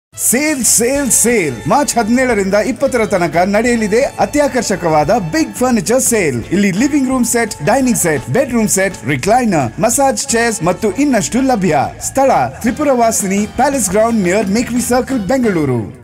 Big Furniture Expo 17th to 20th March - 23 @ Tripuravasini Palace Grounds, Mekhri Circle, Bengaluru
Visit us for an exclusive range of teakwood carved furniture, ethnic furniture, leather sofas, upholstered sofas, water fountains, carpets, metal artifacts, dinning tables, massage chairs, paintings, wall decors, furnishings, wallpapers, seesham furniture, mattresses and much more